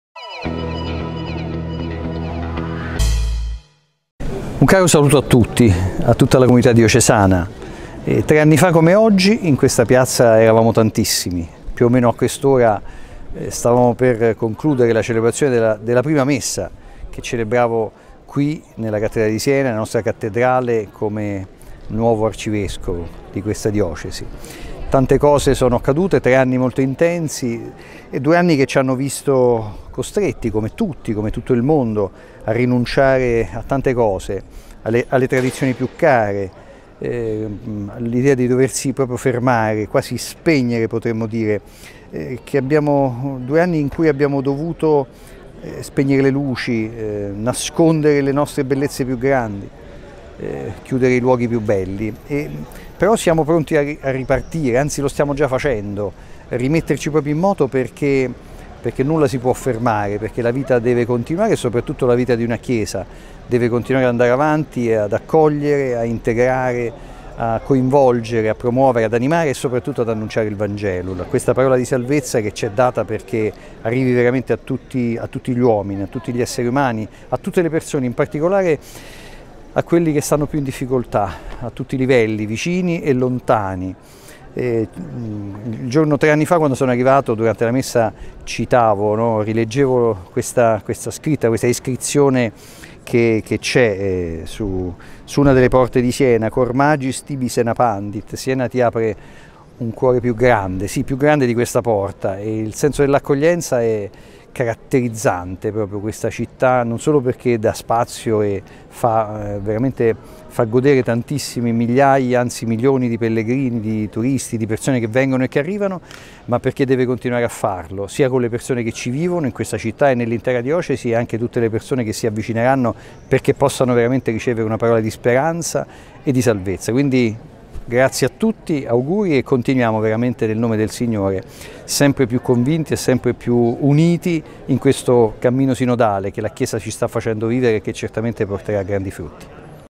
Lojudice, tre anni fa l'ingresso in Diocesi: "Siena continui ad essere accogliente"
"Quando mi avvicinai a questa città lessi queste parole: Cor magis tibi Sena Pandit. Questo significa che il senso dell'accoglienza qui è caratterizzante. E Siena deve continuare ad essere accogliente sia verso chi vuole avvicinarsi all'Arcidiocesi, sia verso chi ha bisogno di un parola di speranza e di salvezza"

Lo ha detto l'Arcivescovo di Siena Augusto Paolo Lojudice in un messaggio alla comunità in occasione del terzo anniversario dell'ingresso in diocesi. "Tante cose sono accadute in questi anni", dove la pandemia "ci ha fatto nascondere le cose più belle, spegnere le luci, rinunciare alle tradizioni", ha aggiunto.

Ma per Lojudice "ora siamo pronti a ripartire con la vita e la Chiesa deve tornare a diffondere il Vangelo a tutto gli uomini e donni. E in particolare a chi vive in difficoltà"